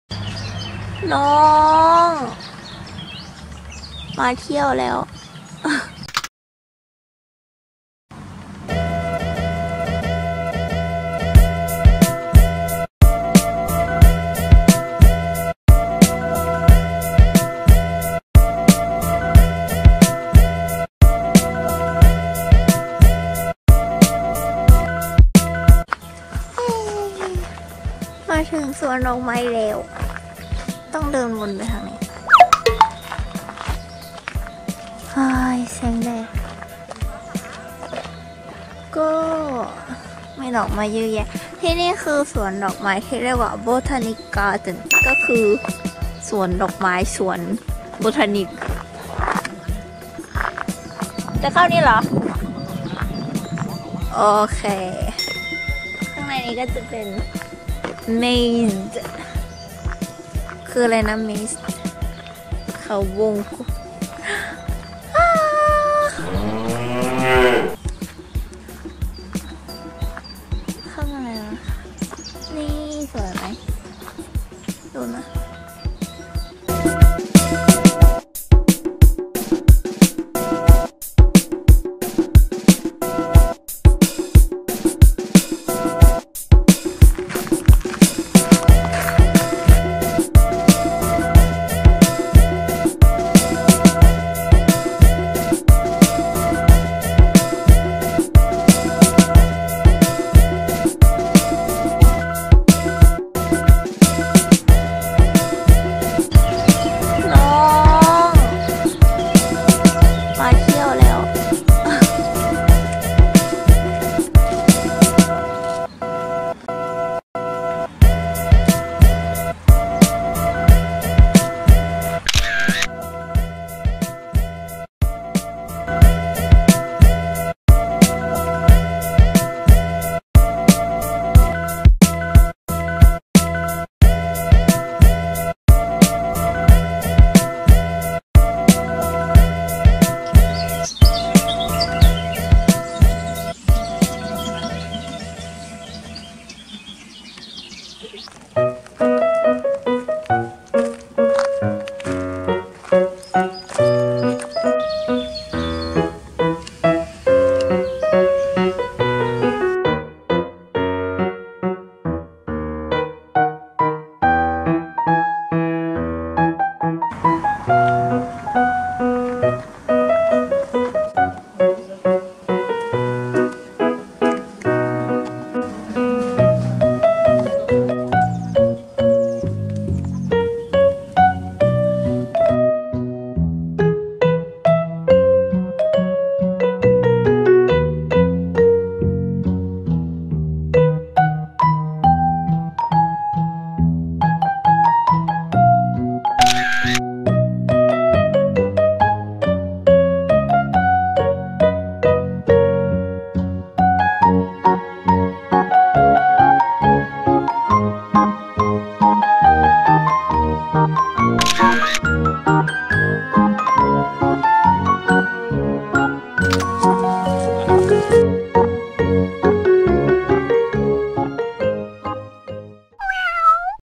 Lukked l VLOG #3 Botanischer Garten Linz 2019 ดอกไม้สวยๆเยอะมาก
A trip to Botanischer Garten Linz 2019

ไปดูดอกไม้สวยๆเยอะแยะเลยค่าาา